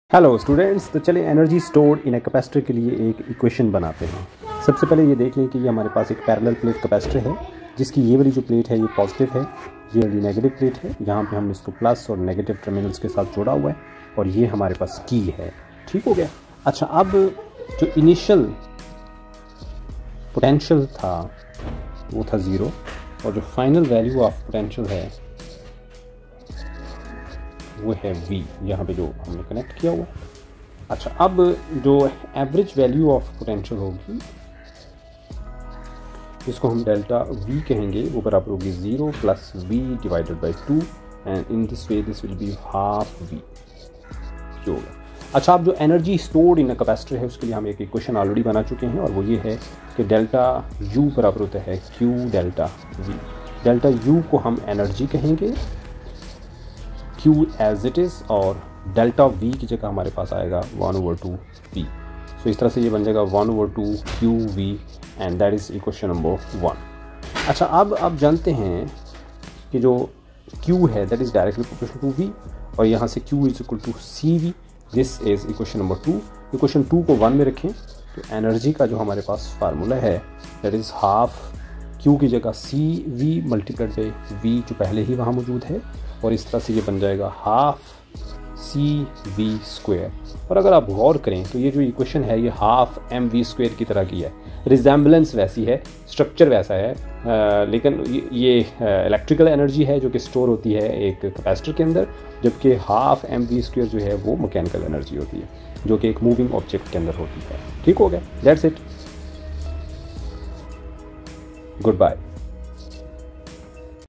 Energy Stored in a Capacitor | Electrostatics | Physics Class 12
In this video, we have shortly derive the equation for the energy stored in a capacitor.
Regards
M Aftab